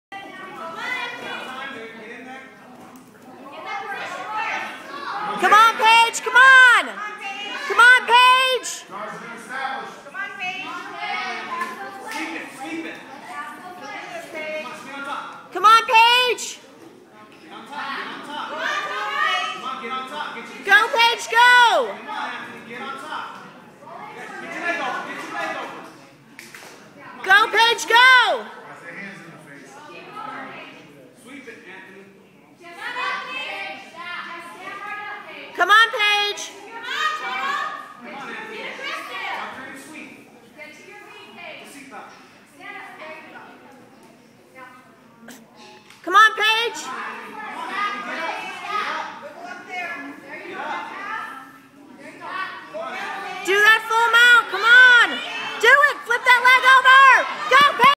Paige vs Anthony 1st place match round 5 part 1 Aug 2013
Fifth round of the tournament. Paige made it to the 1st place match against Anthony. This is part 1 of 2.